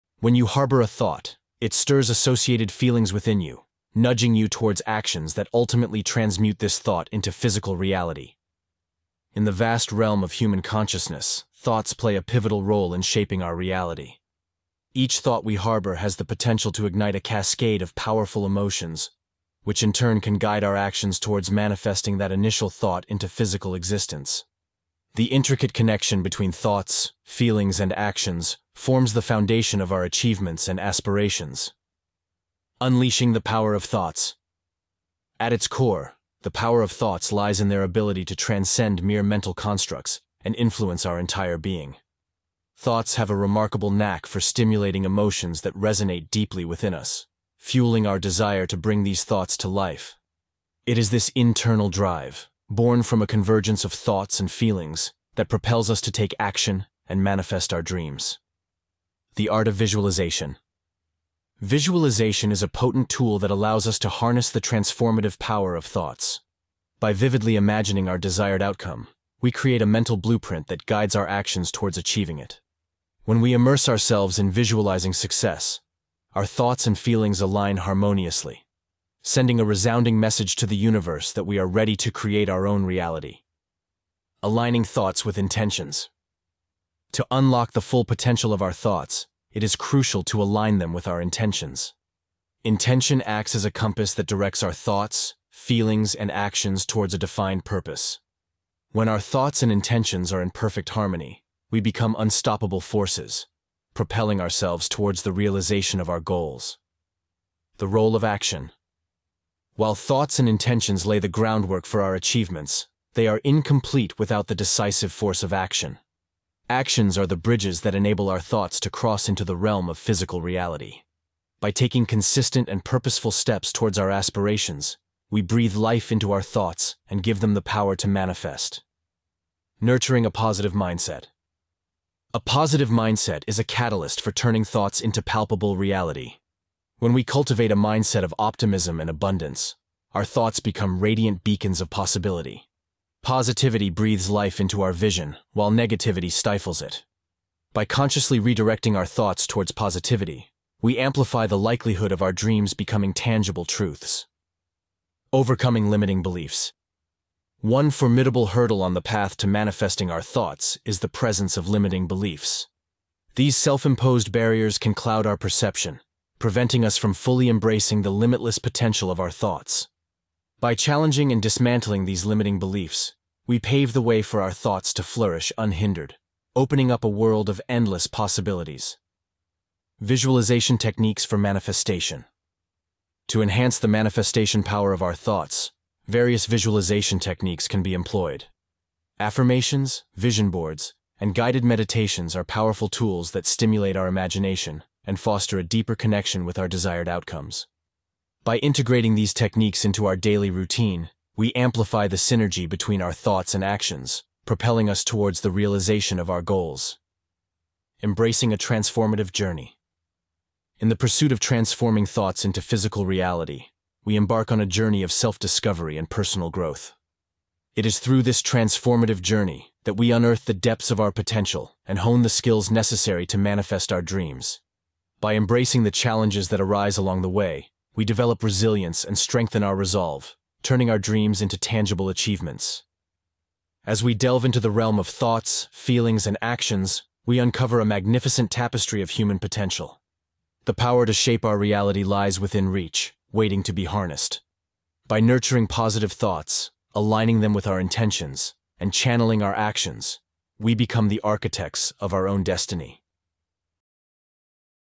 Transform Thoughts to Reality: Visualization Mastery Unveiled! 🌟🧠
🌟 Transform Thoughts to Reality: Unlock Your Visualization Mastery! 🌟

Ever wondered how the power of your thoughts can shape your reality? In this video, we unveil the secrets of transforming your thoughts into tangible achievements with the art of visualization mastery! Discover the intricate connection between thoughts, feelings, and actions and how they can be harnessed to manifest your deepest desires.

- The pivotal role of thoughts in shaping our reality
- How emotions and actions are intertwined with our thoughts
- The transformative power of visualization
- Techniques to align thoughts with intentions for unstoppable momentum
- The importance of nurturing a positive mindset and overcoming limiting beliefs
- Effective visualization techniques for manifestation

Join us as we dive deep into the realm of human potential, and show you how to become the architect of your own destiny. Whether you're looking to boost your motivation, embrace positivity, or simply learn new ways to manifest your goals, this video is a treasure trove of insights!

💡 **Why Watch?**
- Gain practical tips to apply visualization in your daily life
- Learn to overcome hurdles that block your path to success
- Enhance your motivation and positivity with proven strategies

We promise a journey of self-discovery and personal growth that will not only inspire you but also equip you with the tools to transform your dreams into reality. So, why wait to unlock your full potential?

- LIKE if you’re ready to harness the power of your thoughts
- SHARE to spread the inspiration

Your thoughts have the power to create wonders. Are you ready to embark on this transformative journey? Let's turn your thoughts into reality, together. ✨